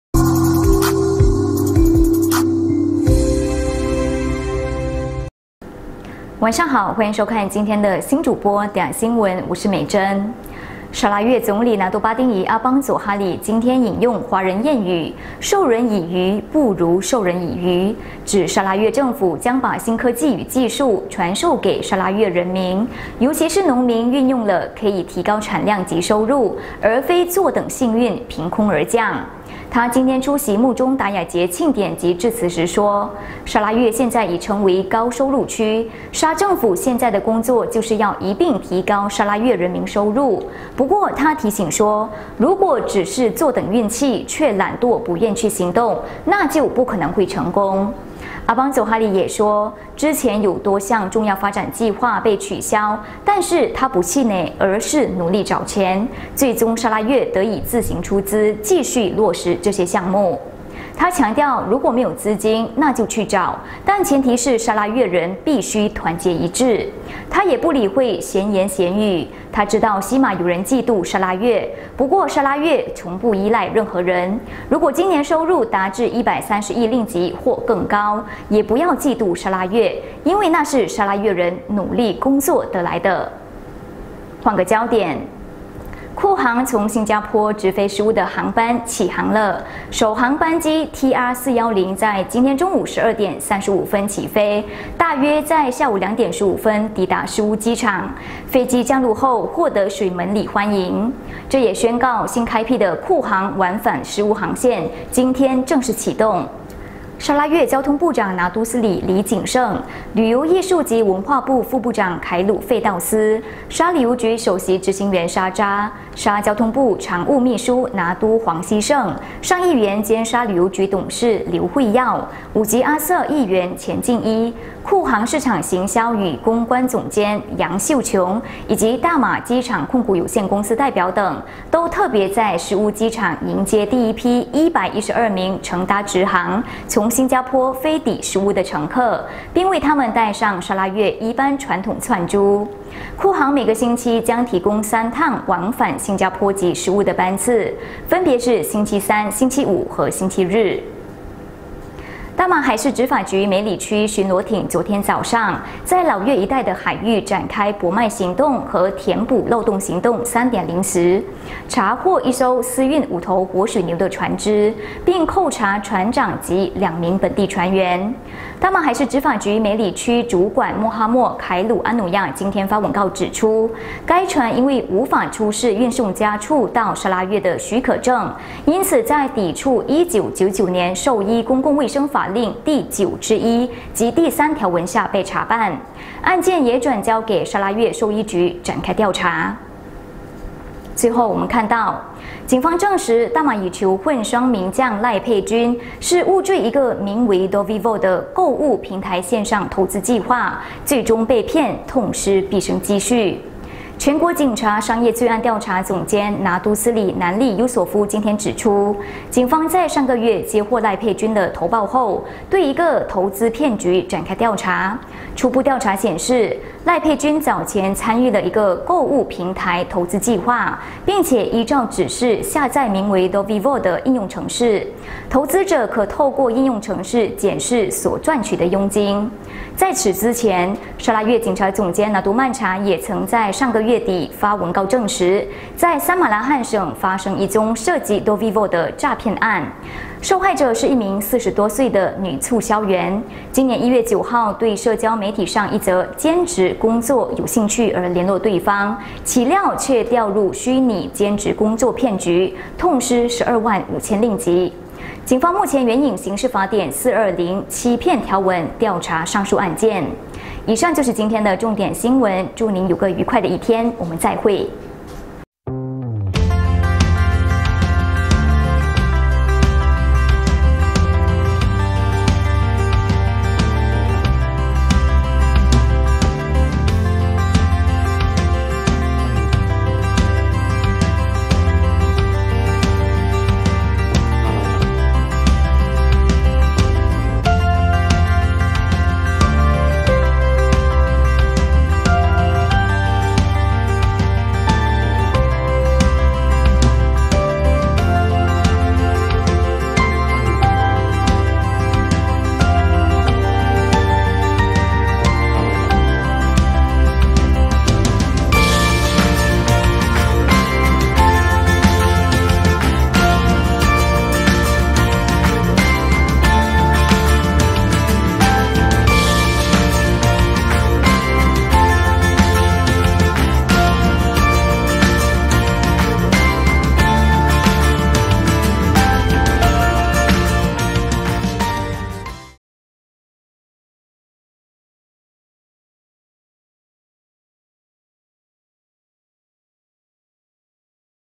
星主播点新闻 6月5日
★采用新技术 非坐等天降 总理：砂要提高人民收入
★新加坡直飞诗巫 酷航首航降陆诗巫机场
★海事局扣3船长及船员 老越海域走私5水牛
★警证实：赖沛君坠“Dobevil”骗局失巨款 砂一女子也中招